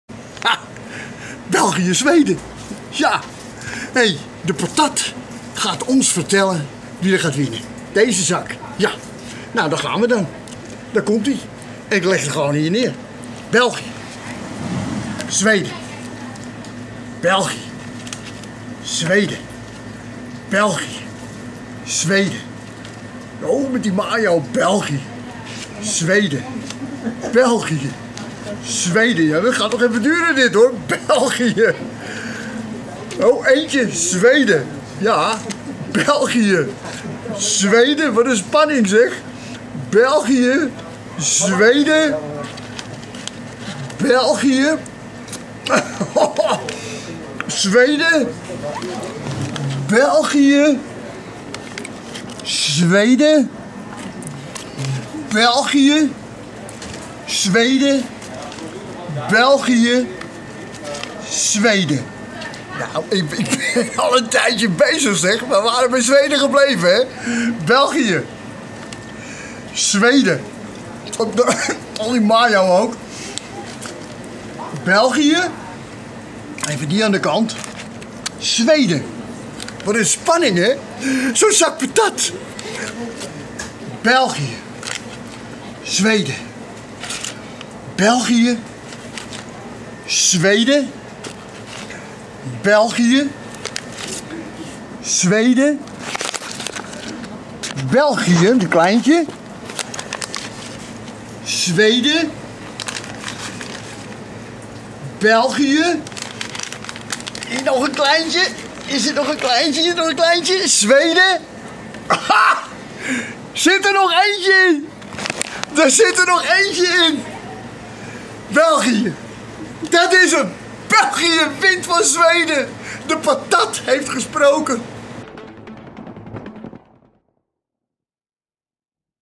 De patat spreekt: 'Wint België of Zweden?'
Wij zijn FF Bellug - maar winnen de Rode Duivels ook van Zweden?? De patat spreekt.